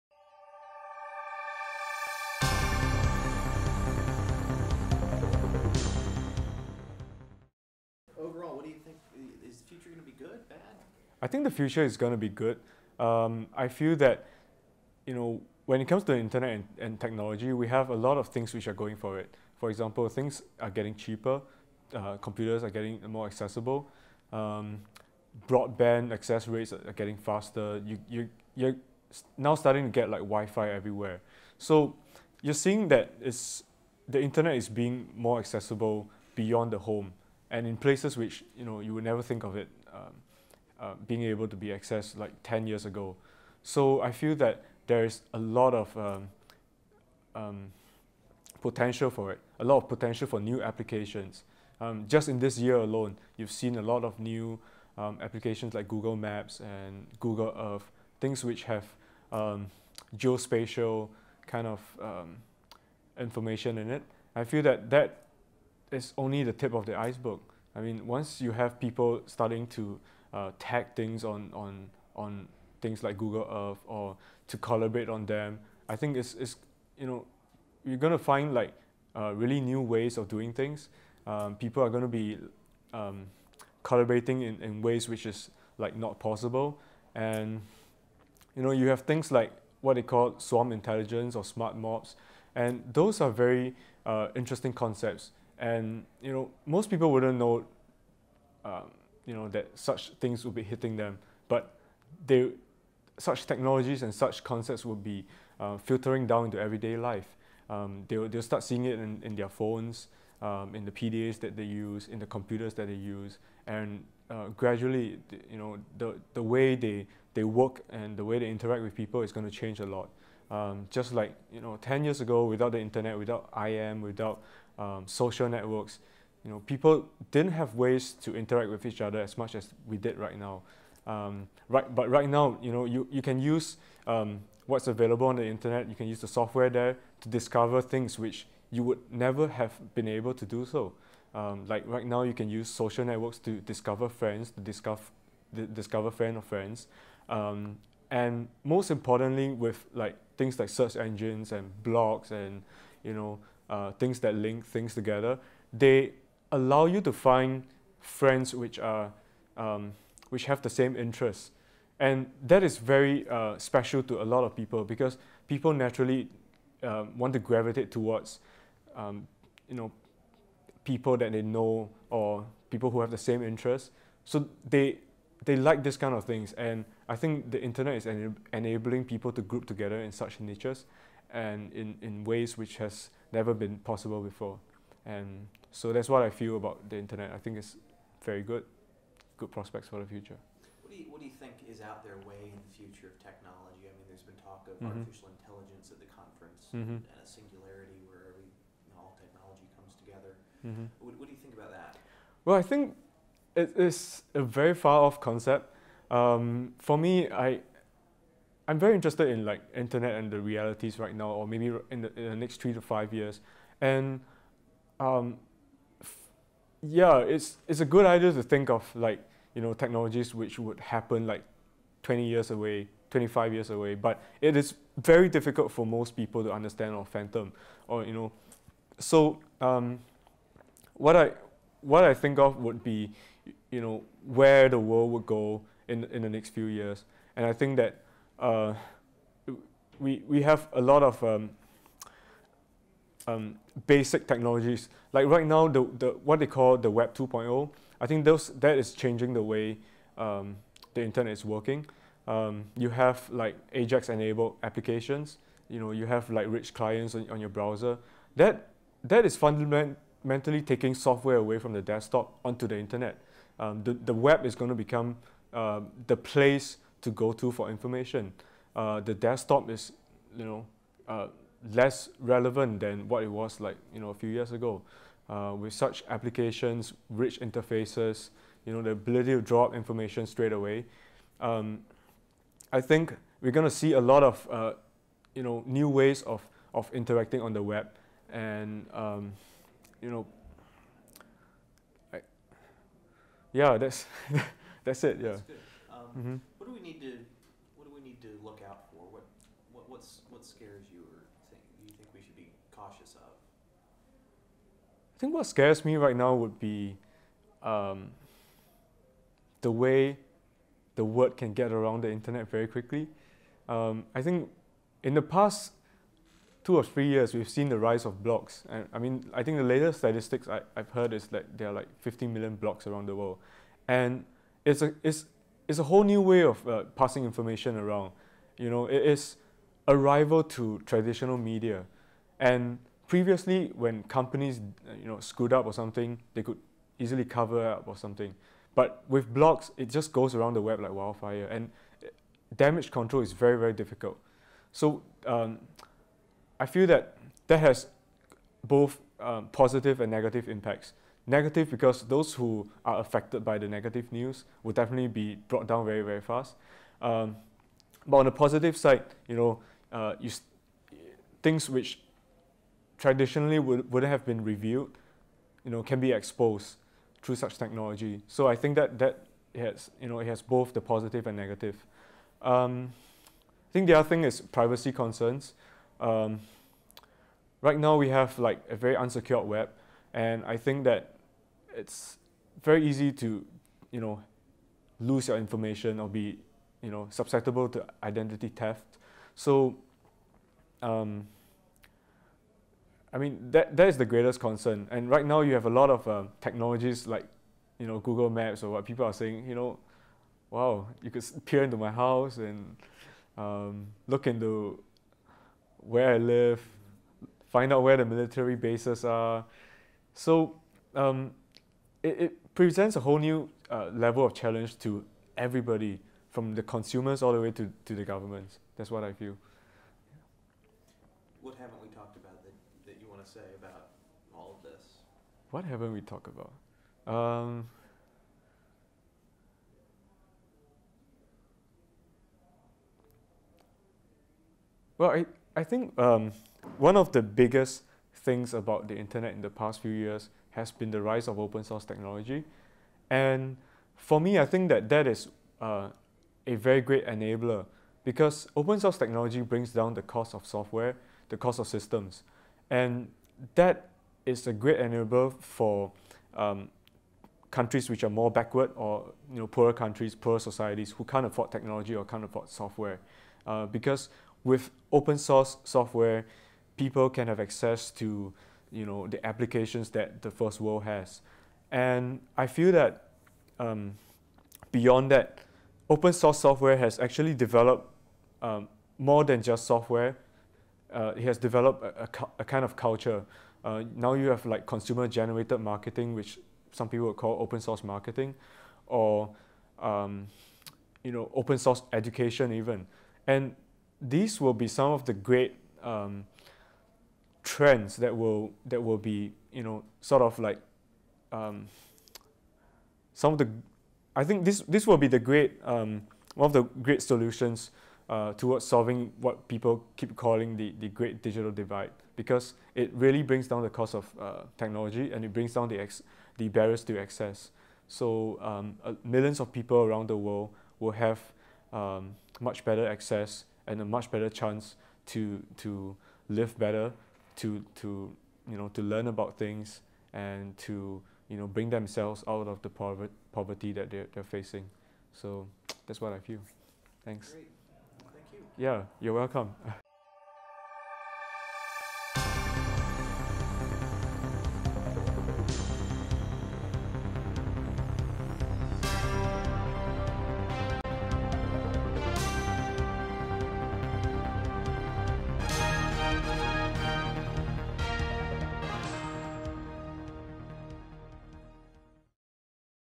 Accelerating Change Conference Interviews, 2005, Palo Alto: Justin Lee
This was recorded at Stanford University in Palo Alto, California, in the spring of 2005 at the Accelerating Change Conference. Recorded by the Imagining the Internet Center, a non-profit, global-good initiative of Elon University, located in North Carolina, USA,